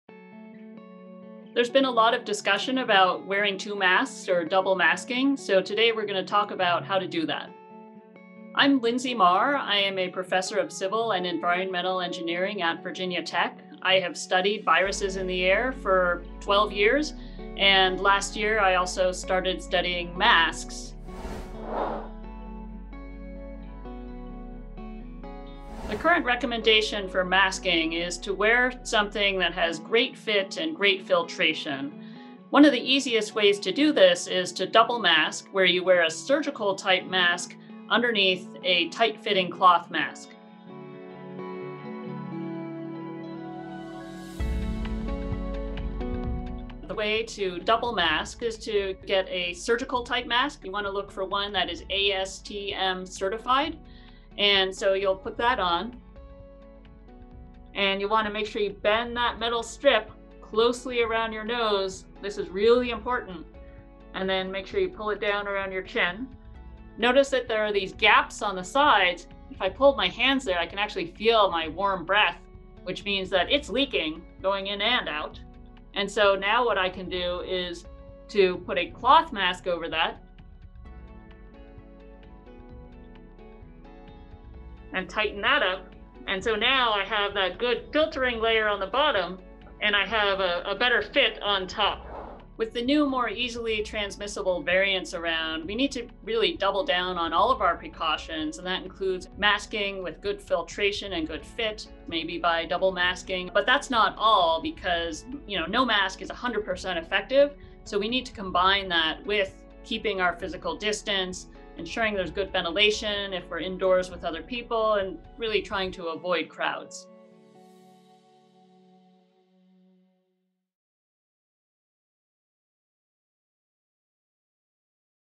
Scientist Explains How and Why to Double-Mask
Wearing two masks can block 92.5 percent of particles. Scientist Linsey Marr shows you how to properly double-mask.